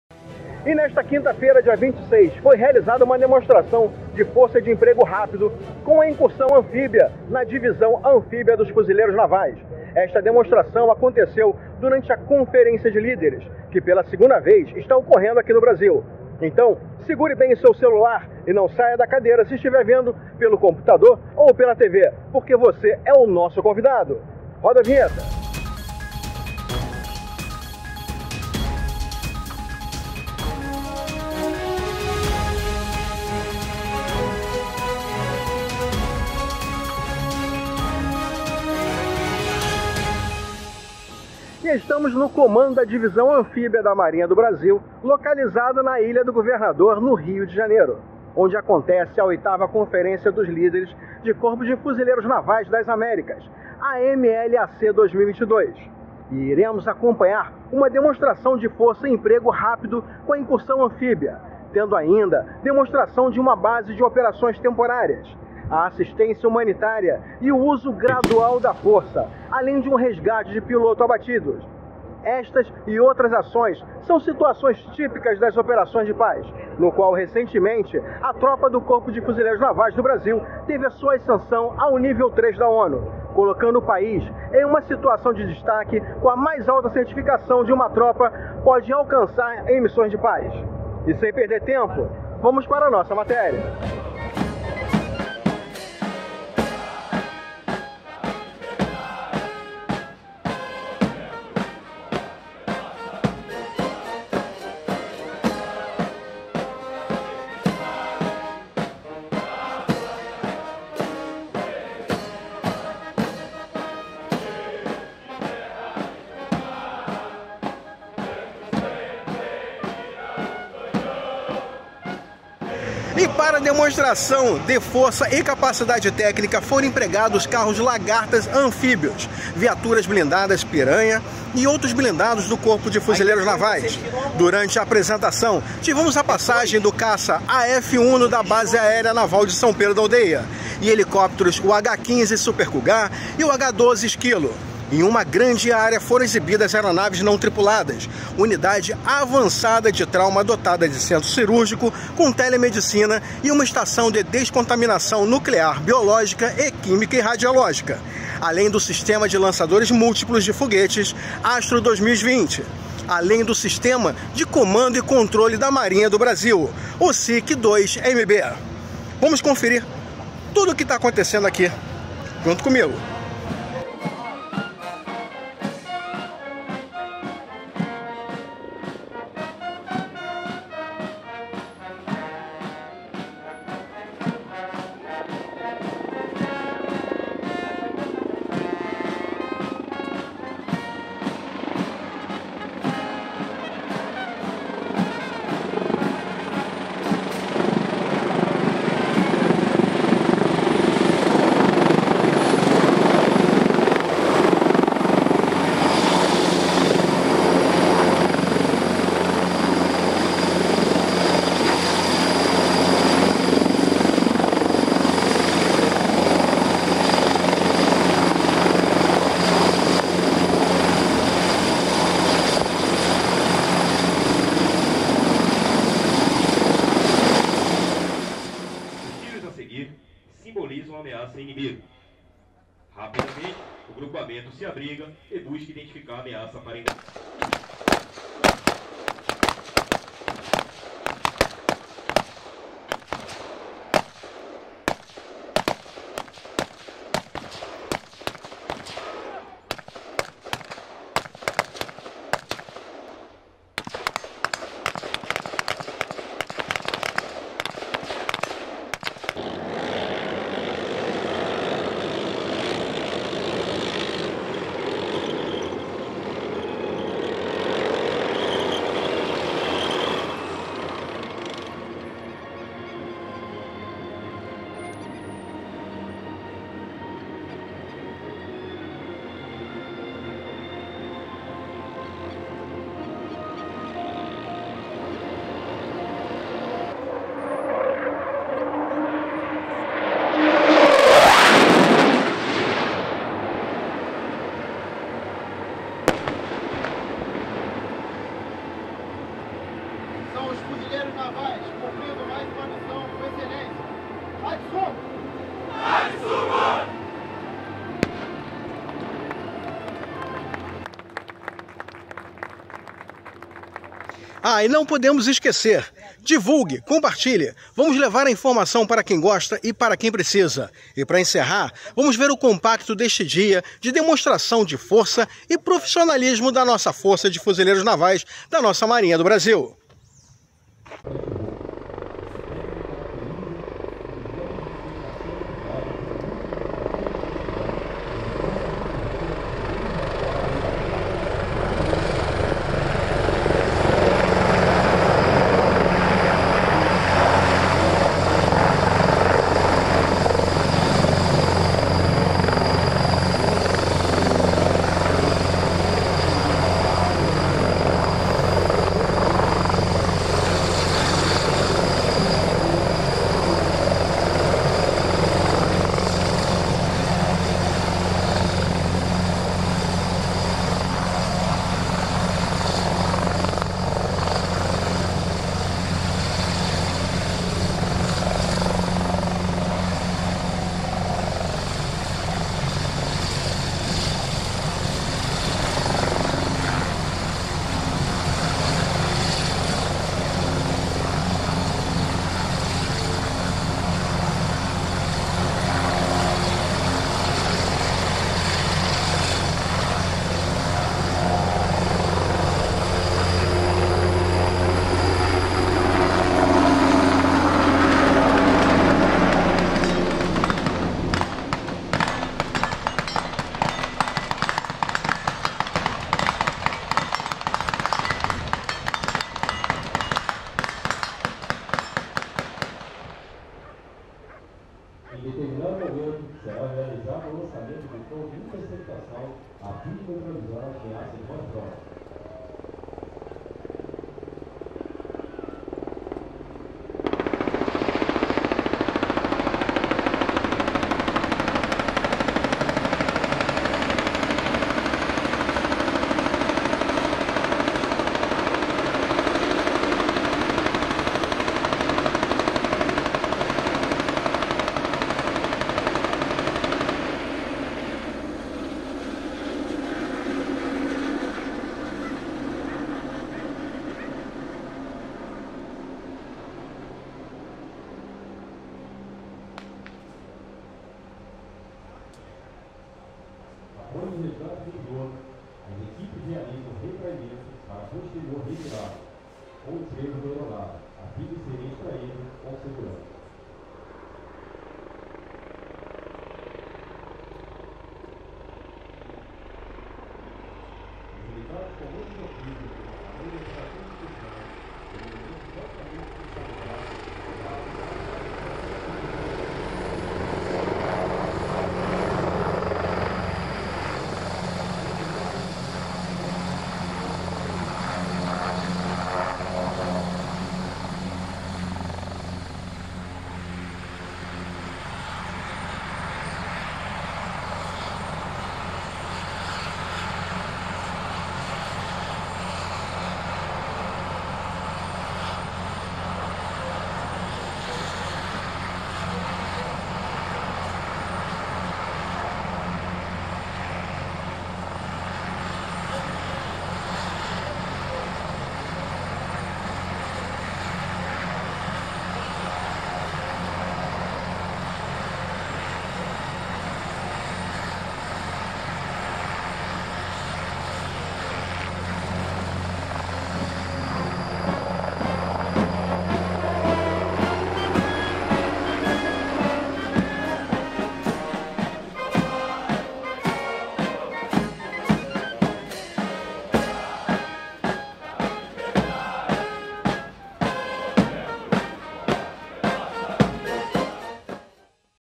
Demonstração Operativa do Corpo de Fuzileiros Navais - Marinha do Brasil
Marinha apresentou suas capacidades aos líderes dos Fuzileiros Navais de 22 países

A Conferência de Líderes dos Corpos de Fuzileiros Navais das Américas (MLAC) tem o propósito discutir soluções modernas, criativas e eficientes para os problemas e questões comuns do hemisfério ocidental. Além da discussão, um dos principais focos é compartilhar experiências entre as instituições dos países envolvidos.

Defesa News é o primeiro programa jornalístico brasileiro com o editorial direcionado para a Base Industrial de Defesa, Segurança e Forças Armadas Brasileiras.

Muito obrigado.